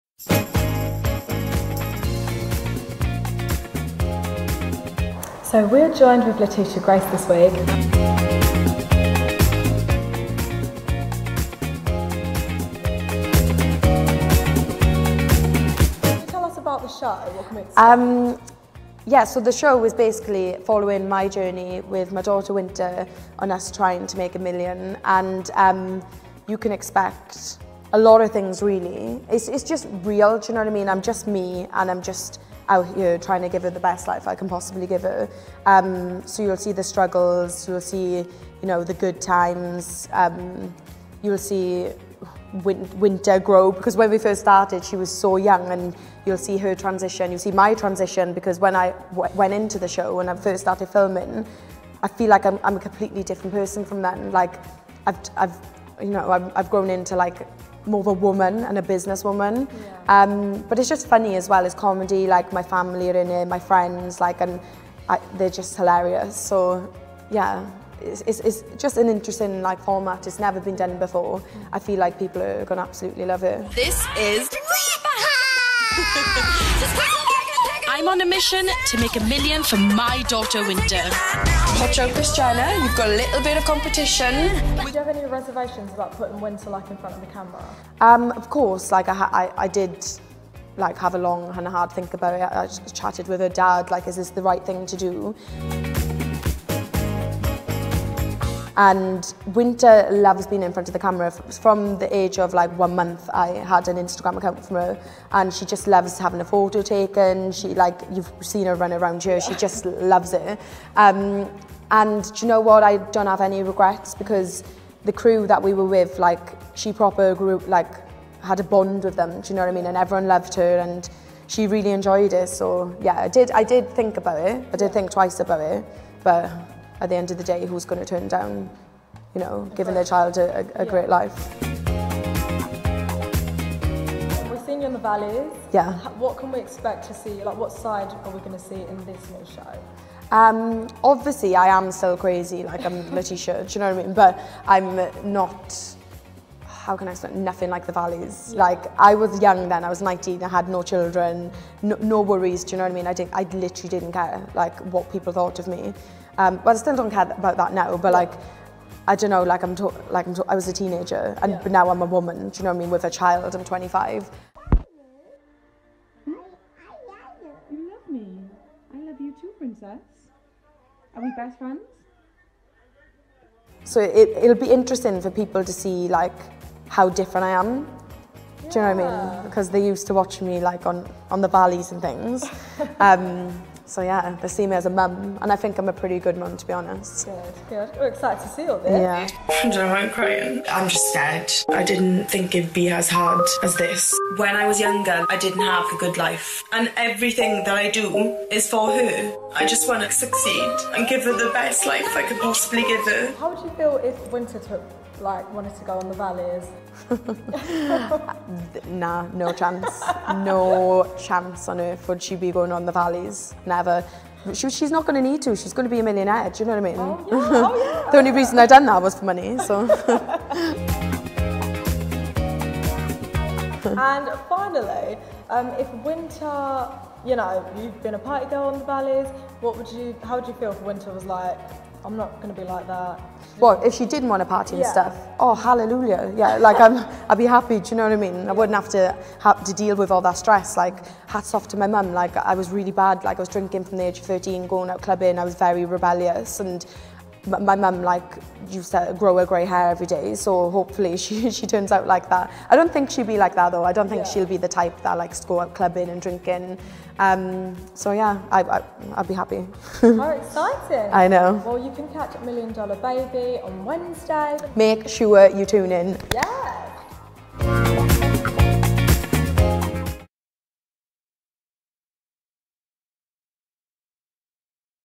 Lateysha Grace reveals ALL things Million Dollar Baby
After talking about the latest celebrity news on Closer Confidential, we sat down with Lateysha Grace to discuss all things about her hit new MTV show Million Dollar Baby.

The former Valleys and Celebrity Big Brother star opened up about life at home with her daughter Wynter and her and her ex-boyfriend's decision on the show.

Million Dollar Baby is on every Wednesday at 8pm on MTV.